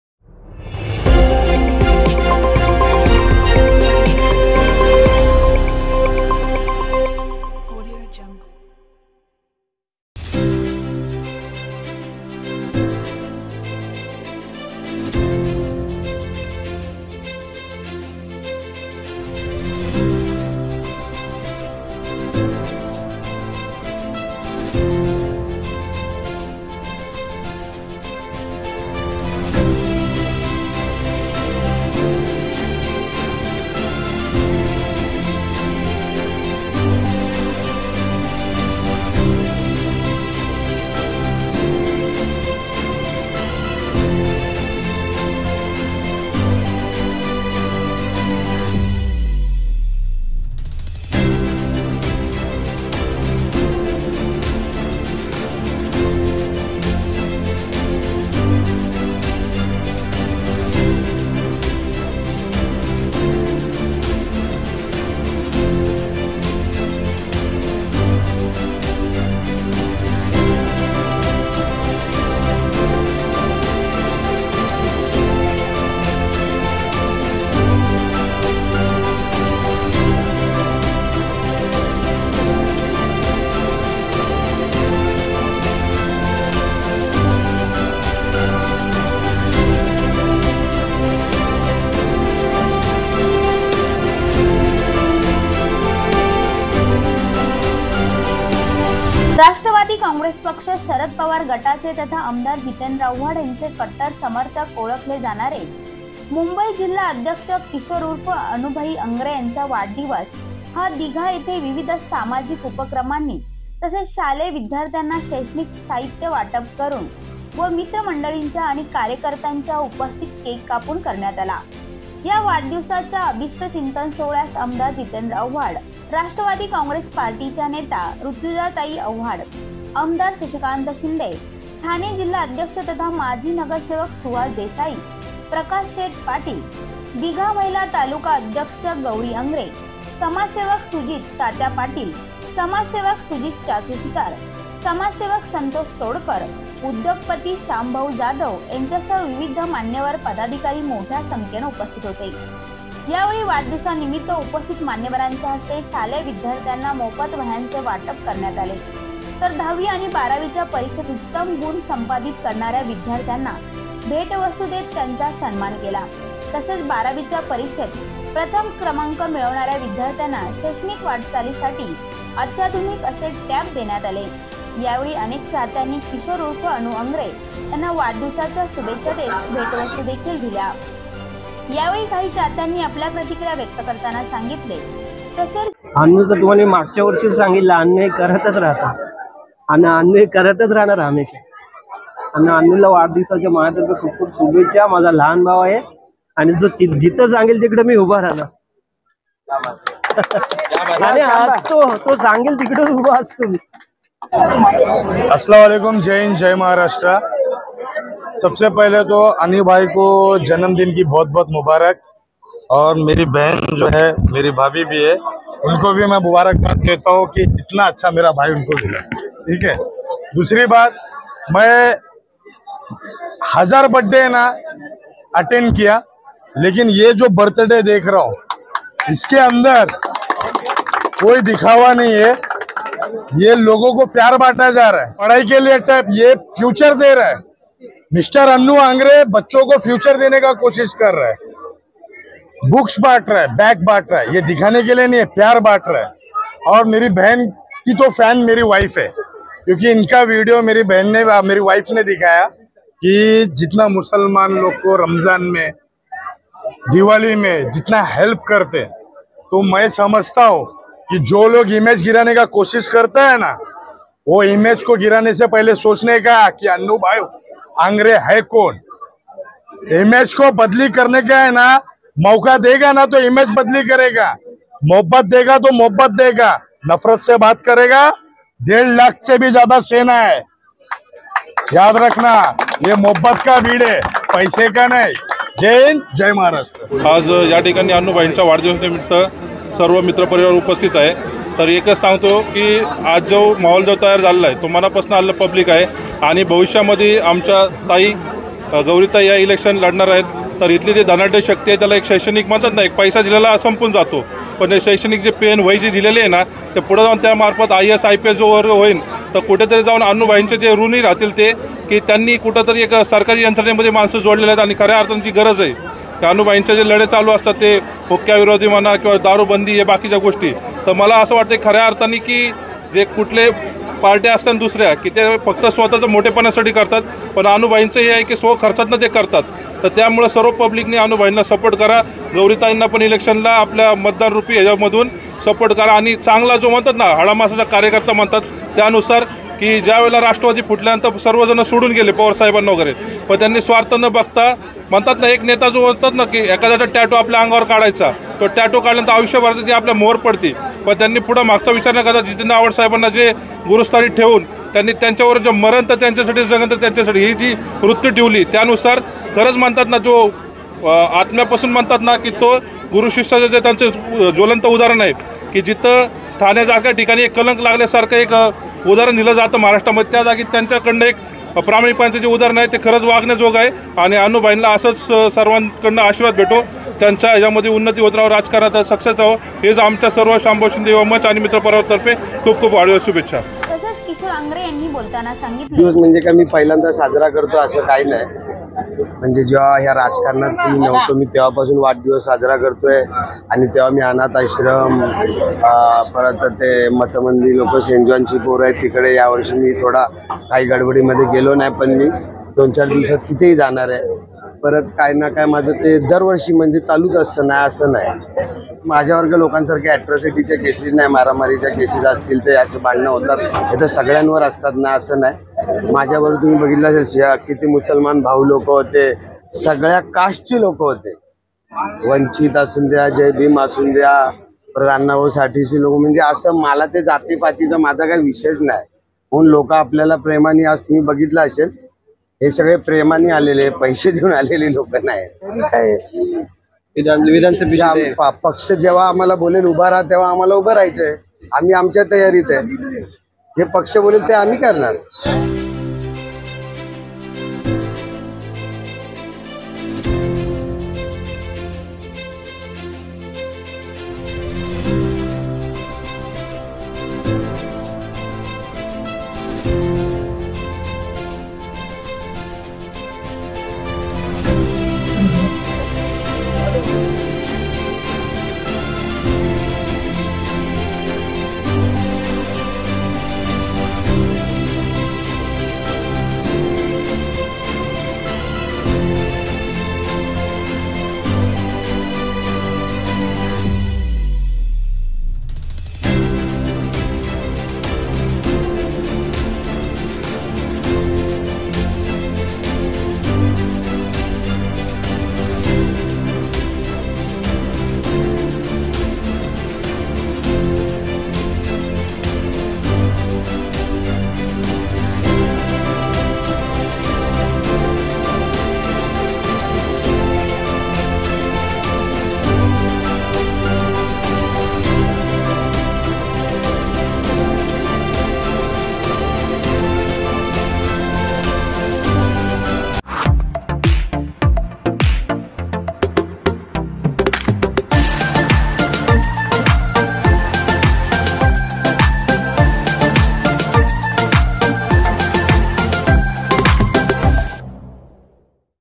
रा.काँ. पार्टी नवी मुंबई जिल्हाध्यक्ष किशोर ( अन्नू )आंग्रे यांचा वाढदिवस सामाजिक उपक्रमांनी साजरा
राष्ट्रवादी काँग्रेस पक्ष शरद पवार गटाचे तथा आमदार जितेंद्र आव्हाड यांचे कट्टर समर्थक ओळखले जाणारे मुंबई जिल्हाध्यक्ष किशोर उर्फ अन्नूभाई आंग्रे यांचा वाढदिवस हा दिघा येथे विविध सामाजिक उपक्रमांनी तसेच शालेय विद्यार्थ्यांना शैक्षणिक साहित्य वाटप करून व मित्रमंडळींच्या आणि कार्यकर्त्यांच्या उपस्थितीत केक कापून करण्यात आला

  या वाढदिवसाच्या अभिष्टचिंतन सोहळ्यास आमदार जितेंद्र आव्हाड, राष्ट्रवादी काँग्रेस पार्टीच्या नेत्या ऋता ताई आव्हाड,, आमदार शशिकांत शिंदे, ठाणे जिल्हाध्यक्ष तथा माजी नगरसेवक सुहास देसाई, प्रकाश शेठ पाटील, दिघा महिला  तालुका अध्यक्ष  गौरी आंग्रे, समाजसेवक सुजित  तात्या पाटील, समाजसेवक सुजित  चाचू  सुतार, समाजसेवक संतोष तोडकर  उद्योगपती शामभाऊ जाधव, यांच्यासह विविध मान्यवर व पदाधिकारी मोठ्या संख्येने उपस्थित होते

 यावेळी वाढदिवसानिमित्त उपस्थित मान्यवरांच्या हस्ते  शालेय विद्यार्थ्यांना मोफत वह्यांचे  वाटप करण्यात आले, तर दहावी आणि बारावीच्या परीक्षेत उत्तम गुण संपादित करणाऱ्या विद्यार्थ्यांना भेटवस्तू देत त्यांचा सन्मान केला, तसेच बारावीच्या परीक्षेत  प्रथम क्रमांक मिळवणाऱ्या विद्यार्थ्यांना  शैक्षणिक वाटचालीसाठी आत्याधुनिक असे टॅप देण्यात आले.